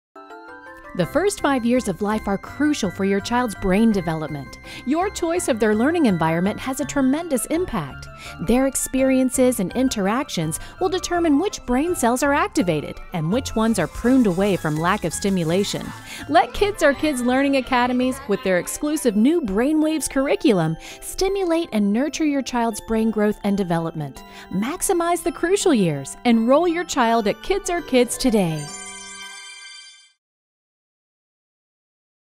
Brain Waves: Key Tasks of Early Brain Development
The first five years of life are crucial for your child's brain development. Your choice of their learning environment has a tremendous impact.

At birth, your child's brain is remarkably unfinished with most of the 100 billion neurons not yet connected in networks. Forming and reinforcing these connections are the key tasks of early brain development and the specific focus of our exclusive Brain Waves™ curriculum.